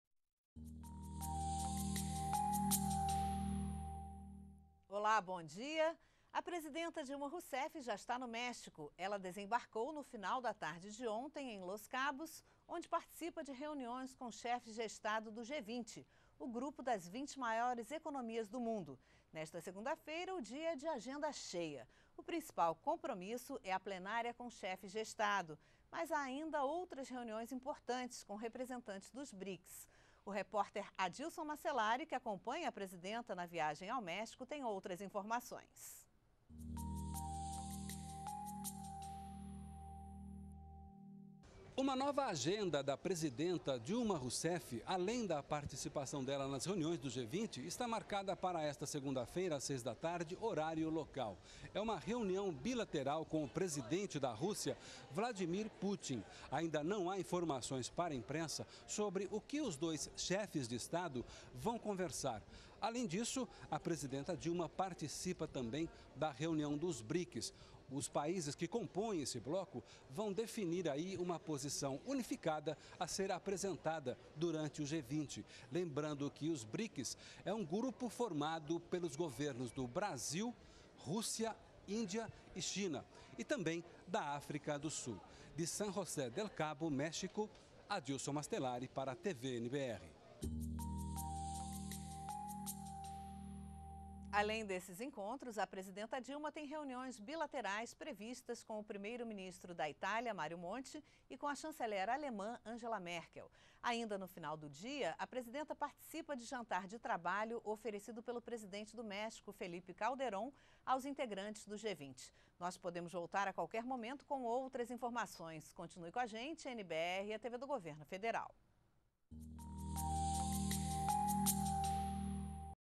Dilma está no México onde participa de reuniões da Cúpula do G20
ATIVIDADES DA PRESIDENTA - 18.06.12: A presidenta Dilma Rousseff já está no México. Ela desembarcou no final da tarde, deste domingo, em Los Cabos, onde participa de reuniões com Chefes de Estado do G20, o grupo das 20 maiores economias do mundo. Nesta segunda-feira (18), o dia é de agenda cheia. O principal compromisso é a plenária com Chefes de Estado. Mas há, ainda, outras reuniões importantes, com representantes dos BRICS, grupo formado por Brasil, Rússia, Índia, China e África do Sul. O reporter Adilson Mastellari, que acompanha a presidenta na viagem ao méxico, tem outras informações.